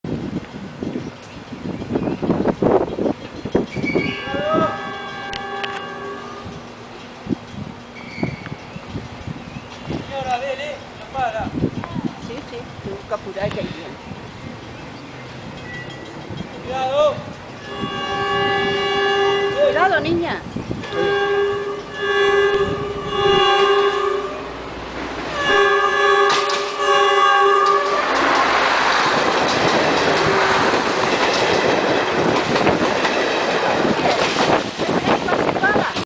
TBA C.A.F. 593 CAMELLO ROSARINO
EL M504 PROCENDENTE DE ROSARIO EN SU VIAJE INAGURAL HACIENDO SU PASO POR LA ESTACION DE VILLA BALLESTER, DONDE UNA SEÑORA FUE APURADA SI NO PERDIA, Y ME DICE JOVEN ESTE ES EL TREN BALA?? Y UNA CHICA QUE ESTABA AL LADO LE DICE: SI! BALA DE AIRE COMPRIMIDO!! CUACK!!